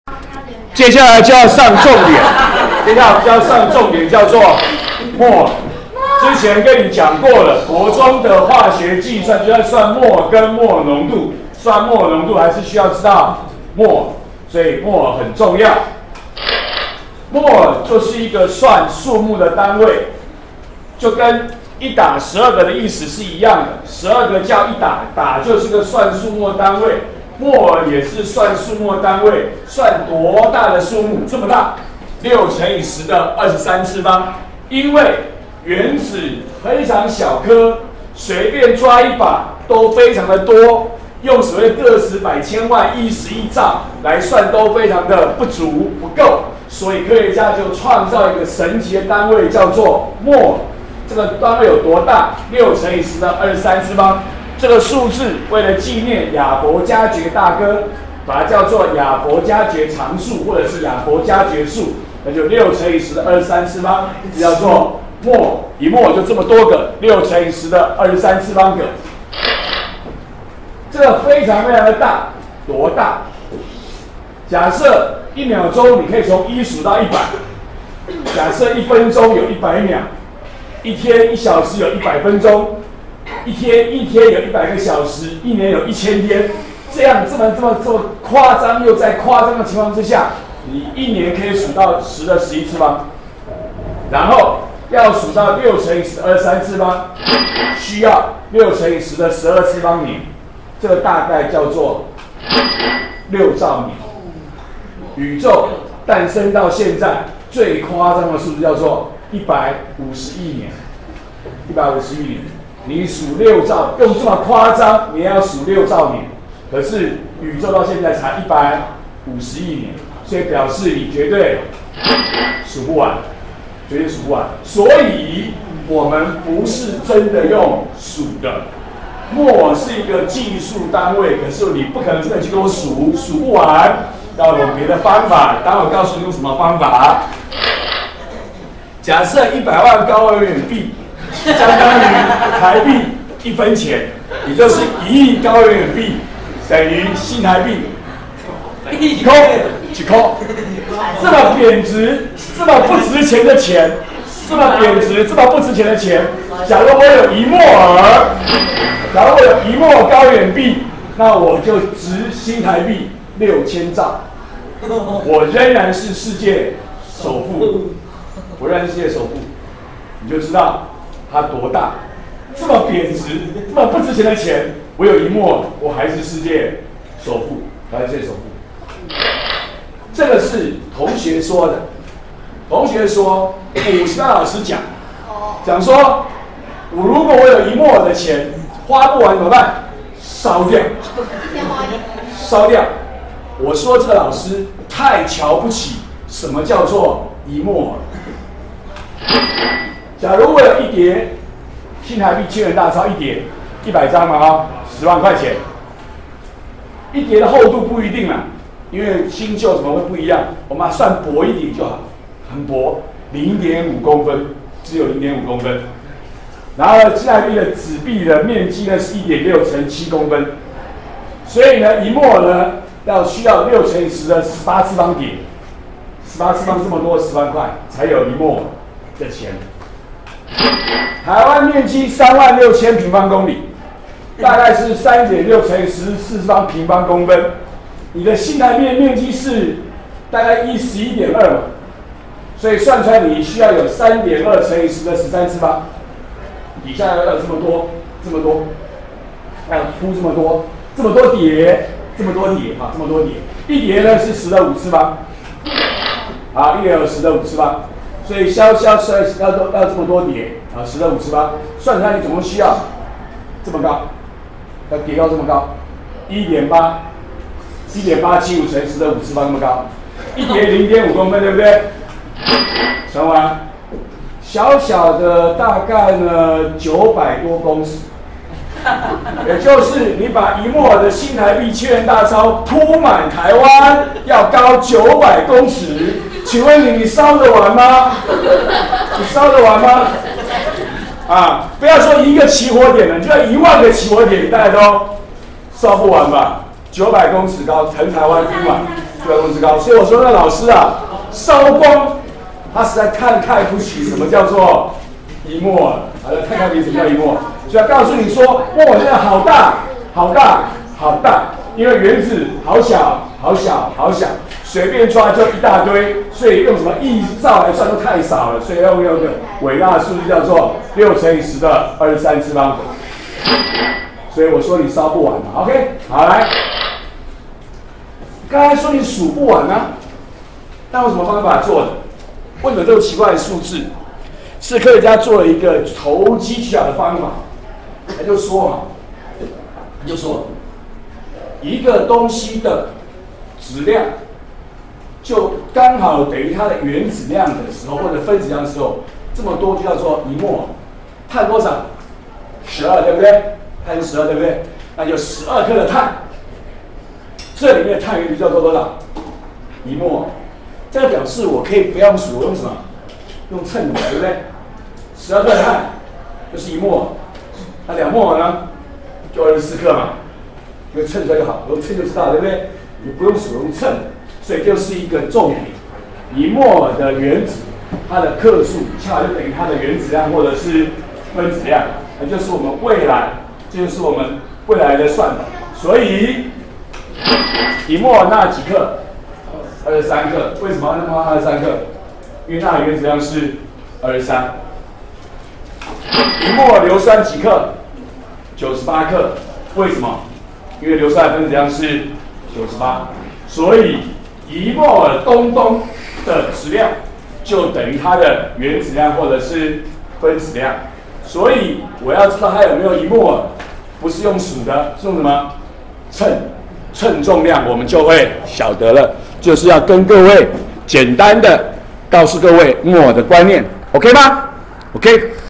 104學年國二理化＿化學反應03：莫耳意義；莫耳與質量
說明何謂莫耳數。以不同例子指出莫耳是個很大的數字。指出莫耳與質量之間的關係。